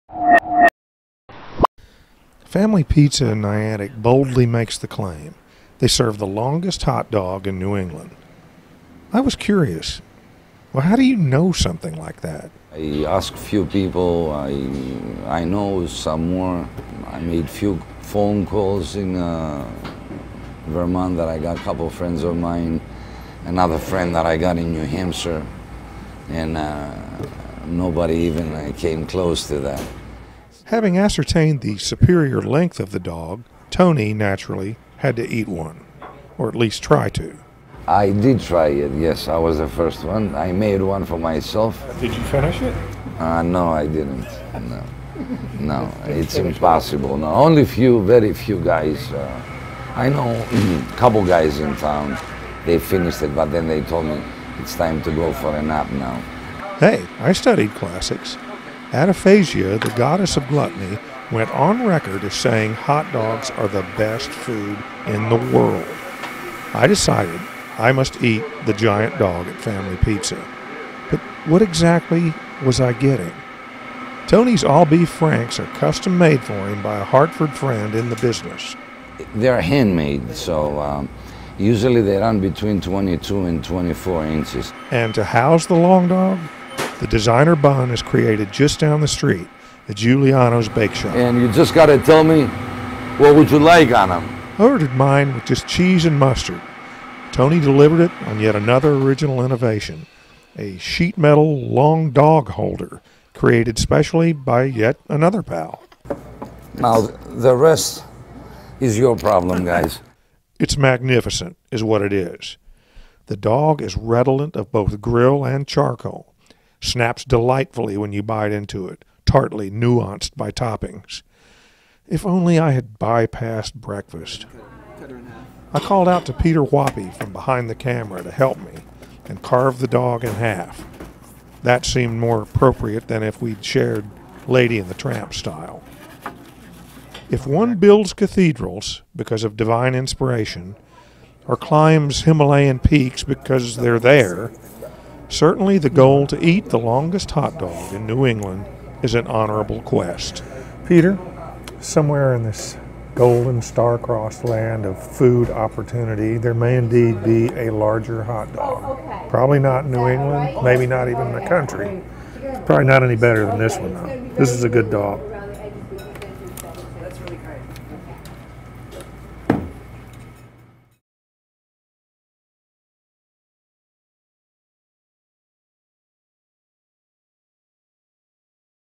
New England's longest hot dog
Family Pizza in Niantic serves a 22-inch hot dog that they claim is New England's longest.